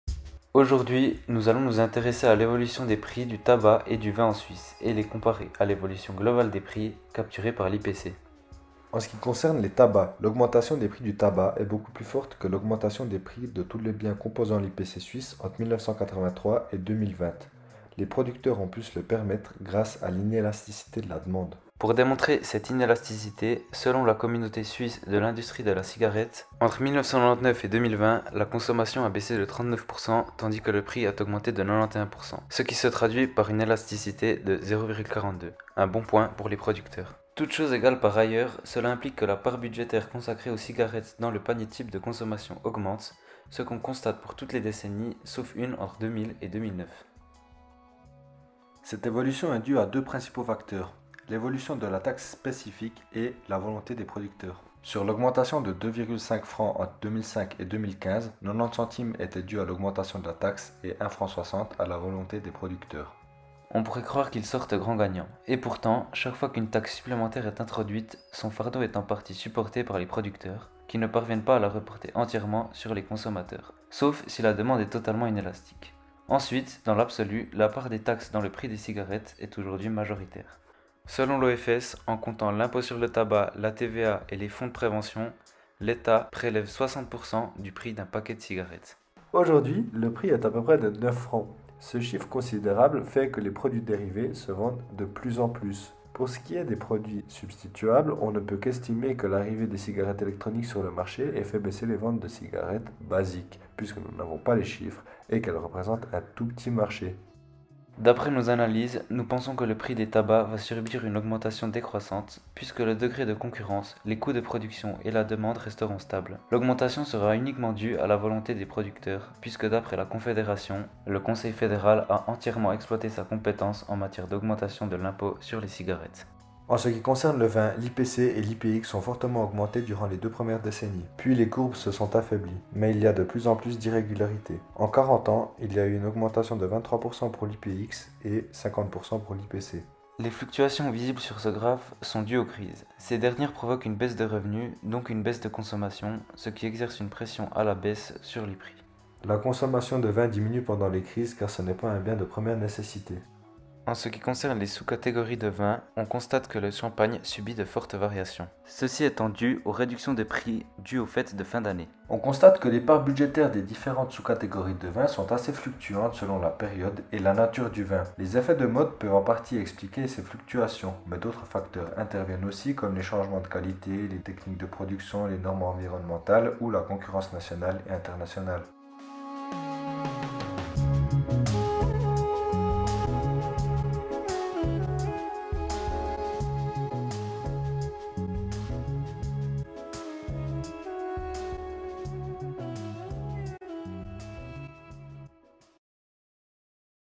L’évolution du prix du vin et des cigarettes sous la loupe
Saviez-vous qu’entre 1999 et 2020, le prix des cigarettes a augmenté de 90%, alors que la consommation a diminué de près de 40% ? Que la consommation de vin a tendance à diminuer en temps de crise, car n’étant pas un bien de nécessité ? Dans cette vidéo scribing, ou dessinée, des étudiantes et étudiants en économie à l'Université de Neuchâtel décryptent de manière ludique l’évolution de l’indice des prix à la consommation du vin et des cigarettes ces deux dernières décennies. Entre analyse et projections futures, elle permet de mieux comprendre les enjeux de ces deux marchés.

Dans le cadre du cours de Micro-économie I du semestre d’automne 2020, les étudiantes et étudiants en économie ont été invité-e-s à vulgariser différents concepts économiques en se basant sur les données de l’Office fédéral de la statistique (OFS). En résulte une série de vidéos pédagogiques visant à analyser l’évolution des indices de prix à la consommation de différents produits.